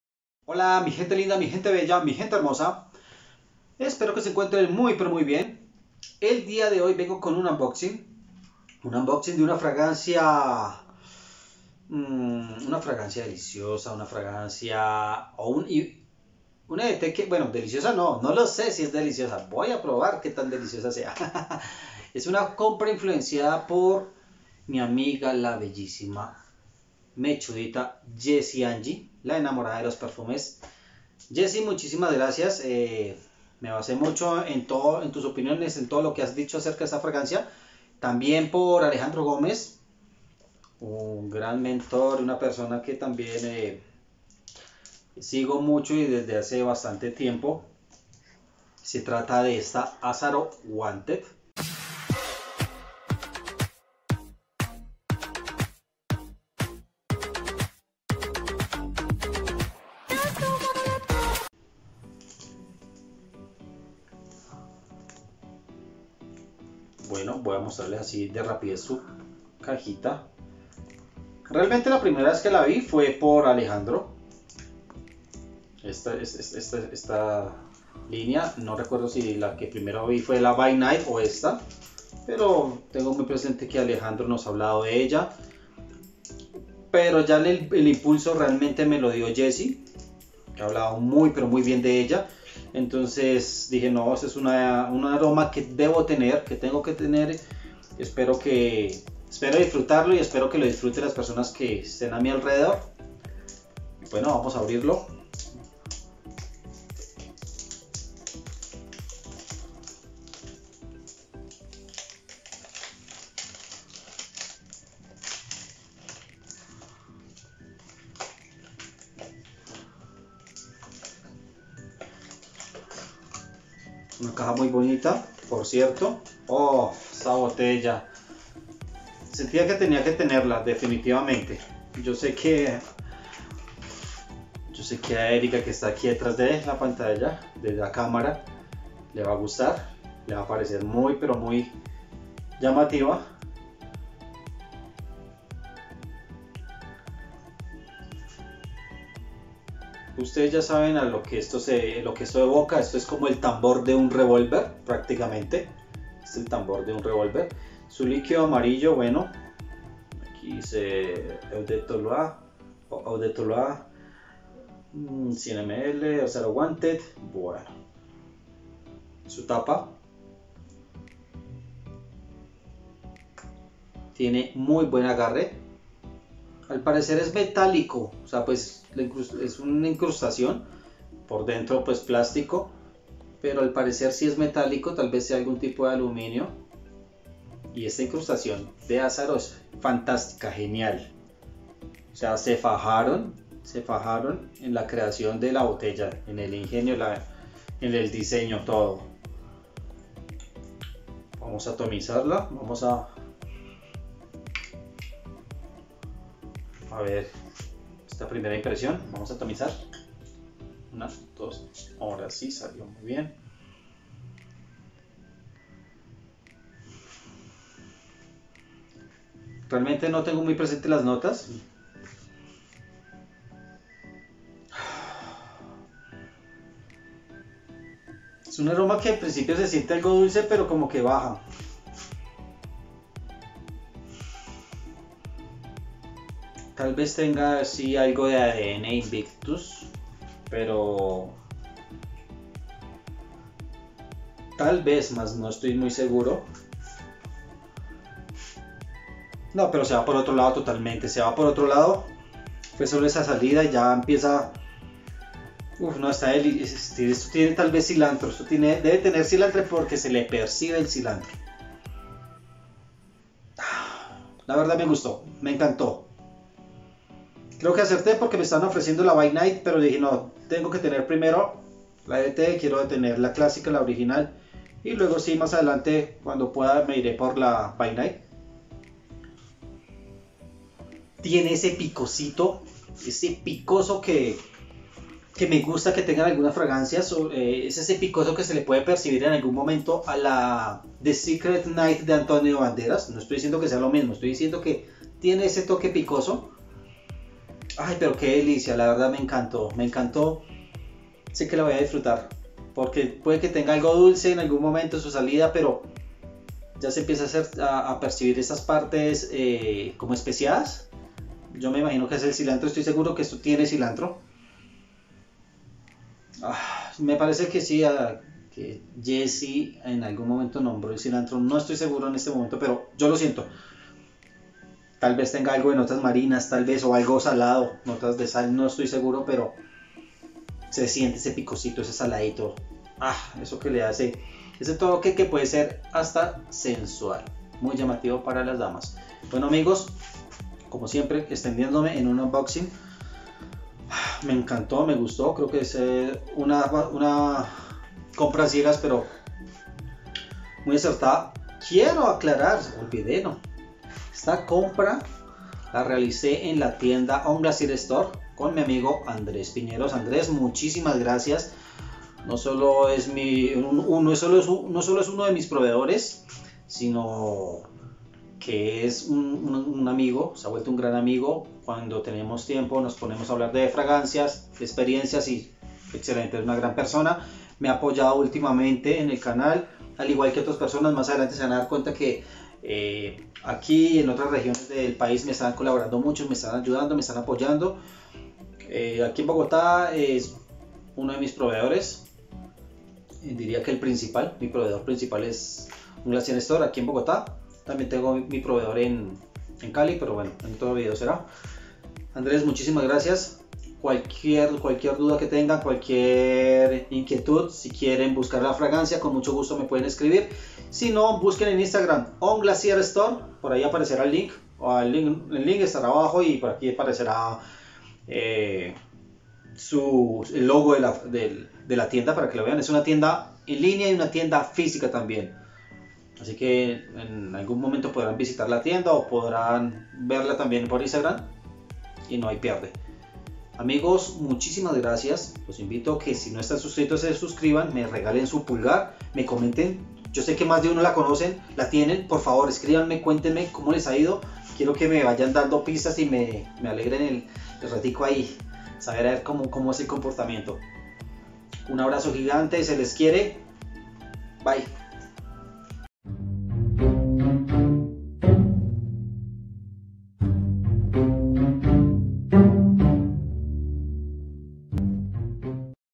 AZZARO WANTED☄️UNBOXING
Hola mis queridos amigos. En esta ocación les comparto este Unboxing de mi primer fragancia de la casa Azzaro.